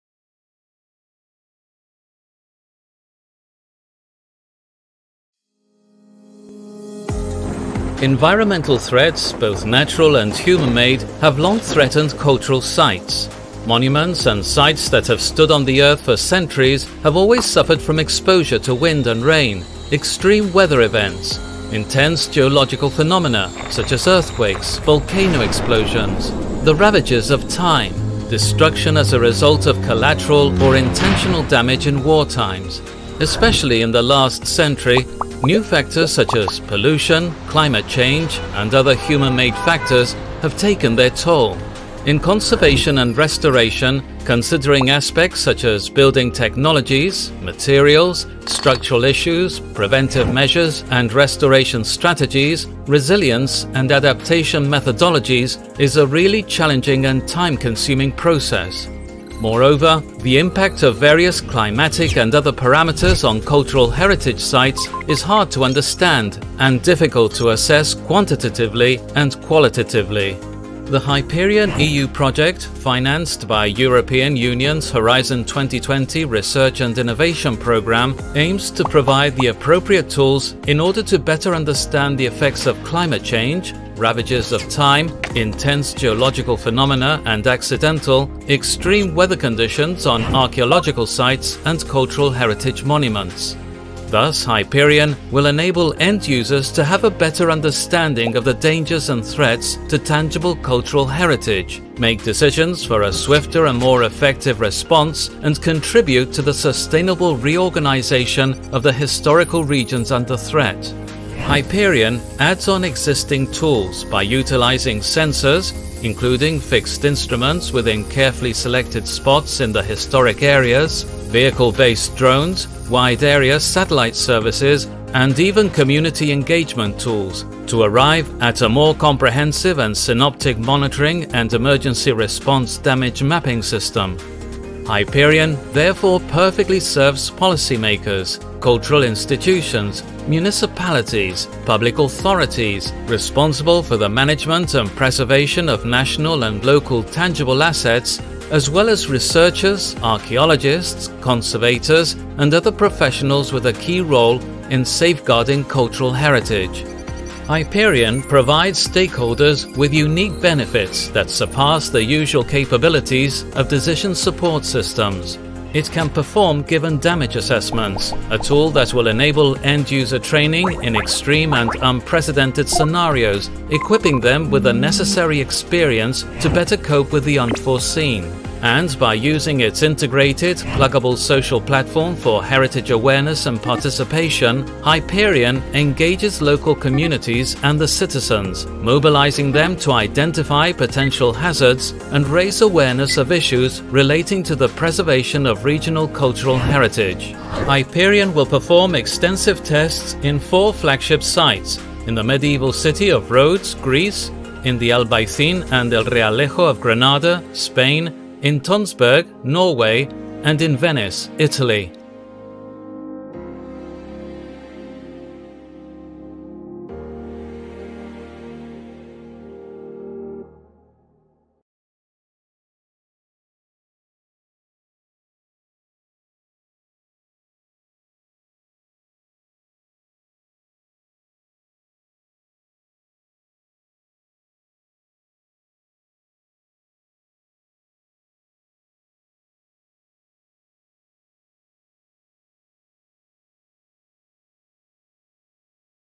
Hyperion EU project video
Recent studies highlight the potential impact of climate change and geo-hazards (such as landslides and earthquakes) on historic areas, hosting cultural heritage sites and monuments, which in turn yield significant adverse impacts on economies, politics, and societies. The deterioration of cultural heritage sites is one of the biggest challenges in conservation; Currently, there is no specific process towards understanding and quantifying climate change effects on historic areas; combined with the limited strategies on climate change-related issues, it becomes difficult to assess quantitatively and qualitatively the impact of various climatic and other parameters on the cultural heritage sites. These issues form an integral part of the necessary support that should be provided to governmental bodies and cultural authorities to properly adapt their policies, in the short and long term, towards deploying sustainable mitigation plans and providing efficient reconstruction of the cultural heritage parts that have been damaged. Finally, the absence of social and humanities and communities’ participatory aspects to the overall resilience and reconstruction planning of the historic areas is the main challenge to tackle. HYPERION takes into account the local eco-systems in the cultural heritage areas, mapping out their interactions and following a truly integrated/sustainable reconstruction approach (technical, social, institutional, environmental and economic level), by incorporating active communities participation and by supporting new business models based on the concept of a “load-balancing” economy, and offering financial risk-transfer tools that can ensure the immediate funds availability to fuel timely build-back-better efforts. HYPERION aims to use existing tools and innovative technologies and create an integrated resilience assessment platform, in which end users will be able to have a better understanding of the dangers and threats of tangible cultural heritage, make decisions for a swifter and more effective response, and contribute to the sustainable reorganization of the historical regions under threat.